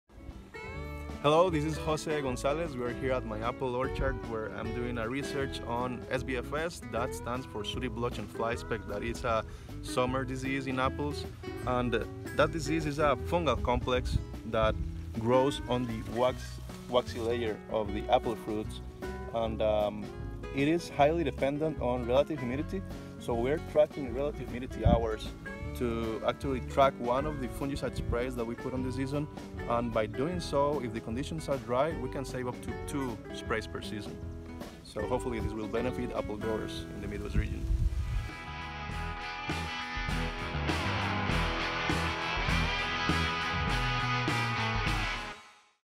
IPM Shorts: Tasty Apples...OH, WAIT! (Sooty Blotch and Flyspeck)
Jose is an Agronomist with a B.Sc. and Licentiateship from University of Costa Rica, with an emphasis on plant pathology, IPM, organic farming, biological control, and business management.
He's a Research Associate at Gleason Lab, and his main role is to oversee the progress of the project's goals and objectives.
He likes to be out in the field, learn from both academia and farmers, and to participate in projects focused on improving sustainable food-production systems.